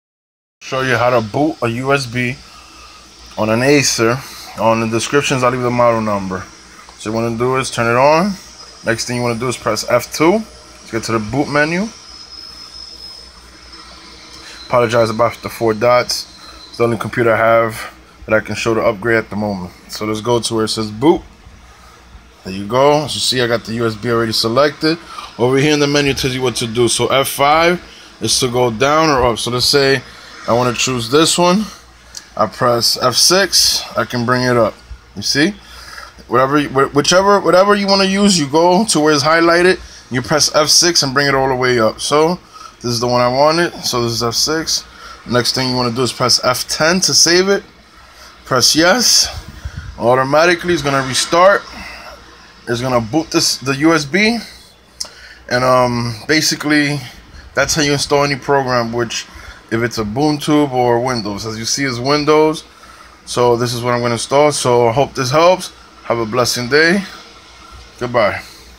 How to boot a usb on a acer aspire 5532 or any acer
A great way to install the latest window if you forget your password or just want a new program install.